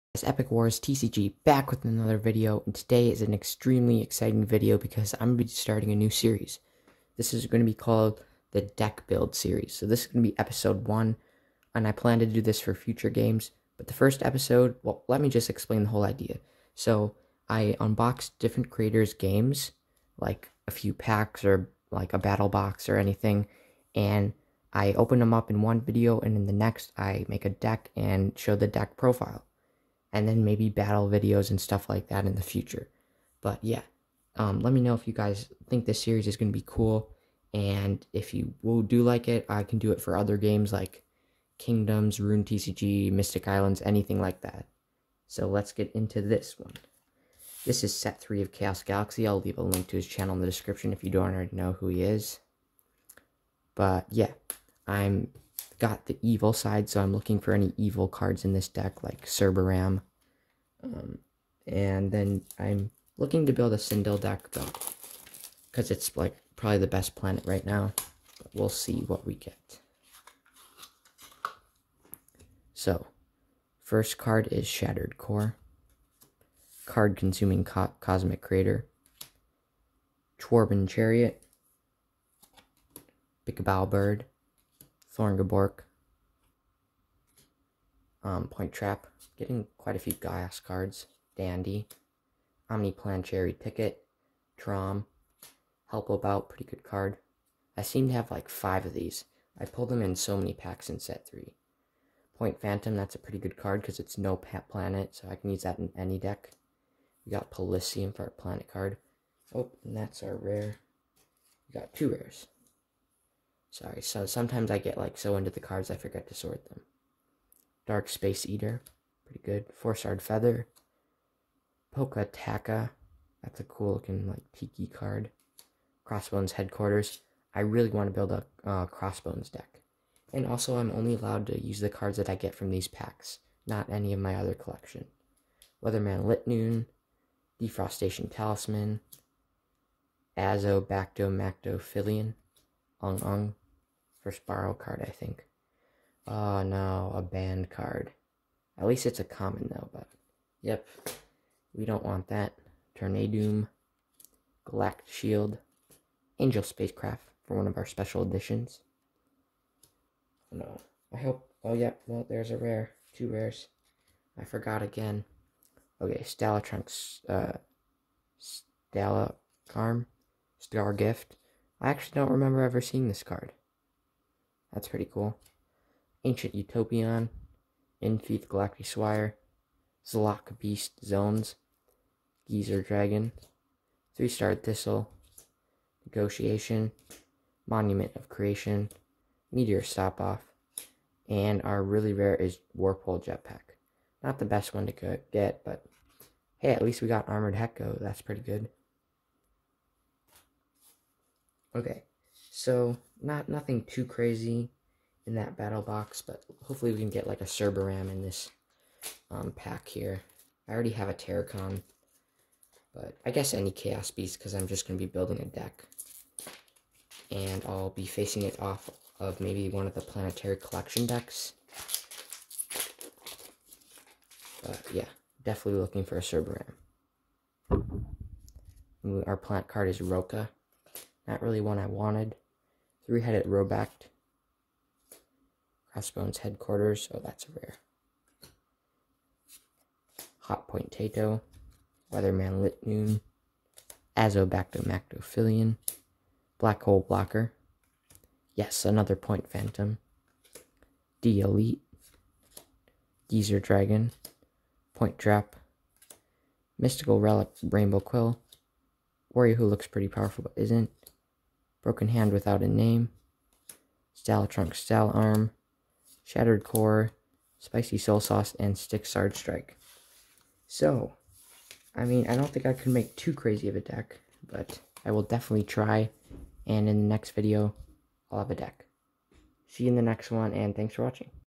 Deckbuild Series #1 ( Chaos galaxy set 3)
this is just a small series showing the cards I pull from some chaos galaxy products, and If I will be able to build a deck out of them too.